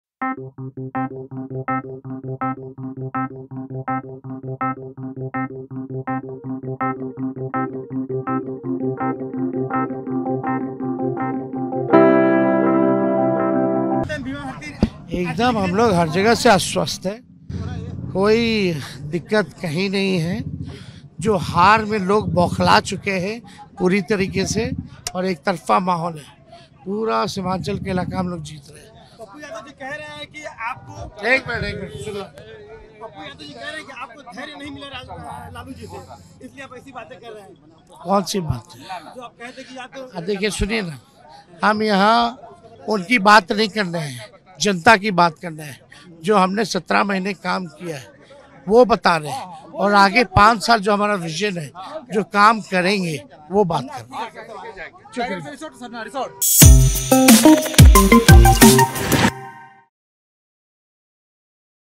Purnia में 'NDA के पक्ष में वोट करने' का बयान देने के बाद Tejashwi Yadav ने निकाला रोड शो
राजद नेता तेजस्वी यादव ने राजद नेता मनोज कुमार झा और विकासशील इंसान पार्टी के नेता मुकेश सहनी के साथ INDIA गठबंधन उम्मीदवार बीमा भारती के समर्थन में रोड शो किया... तेजस्वी यादव ने कहा, "हम लोग हर जगह से आश्वस्त है, कहीं कोई दिक्कत नहीं है। हार में वे लोग बौखाला चुके हैं, एक तरफा माहौल है। पूरा सीमांचल हम जीतने वाले हैं।'